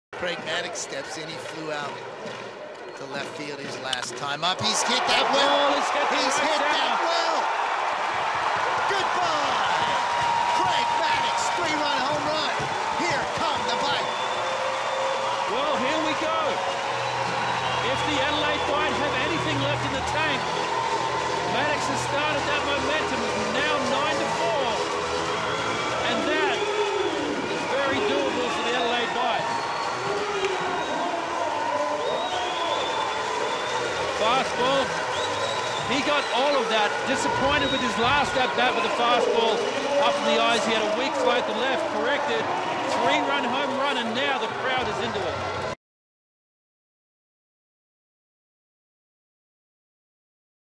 Maddox's three-run homer
2/8/15: Adelaide Bite catcher Craig Maddox launches a three-run homer to deep right-center, cutting the Heat lead to 9-4 in the 4th inning.